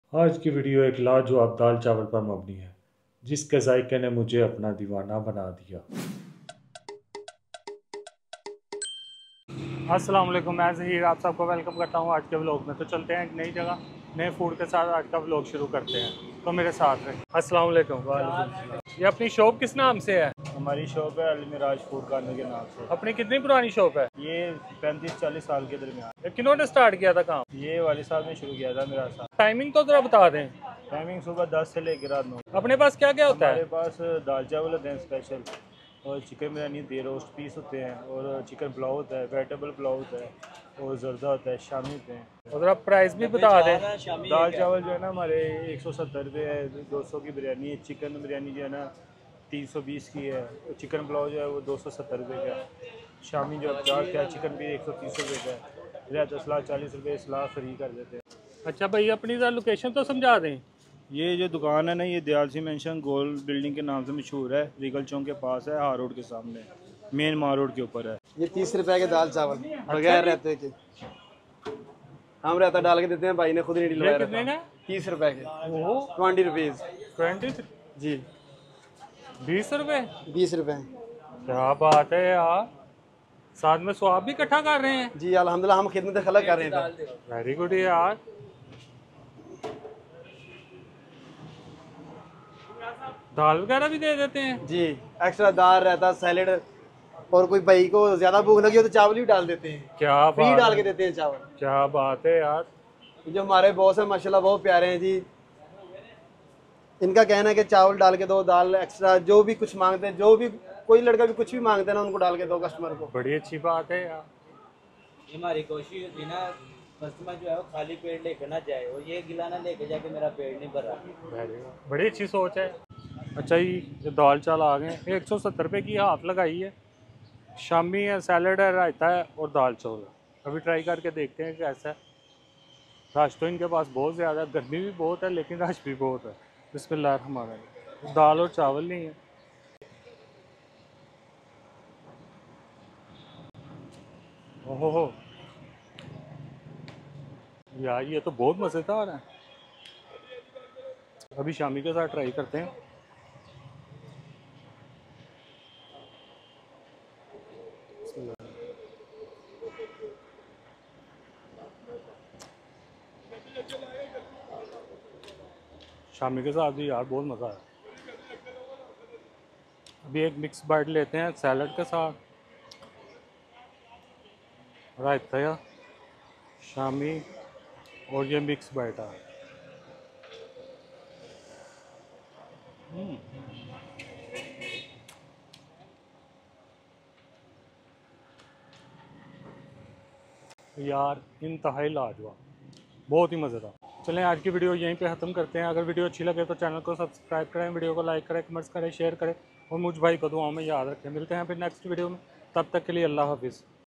Zabardast daal chawal | Best experience this food
Today, I had the chance to experience the famous Me'raj Daal Chawal on Mall Road, Lahore. Their lentil rice was truly delicious! I ordered a half plate which cost only 170 rupees. It included:
- Flavorful lentil rice
- Shami kebab
- Fresh raita
- Crunchy salad

If you want to experience authentic Lahori taste, you must visit this place!

**Location:**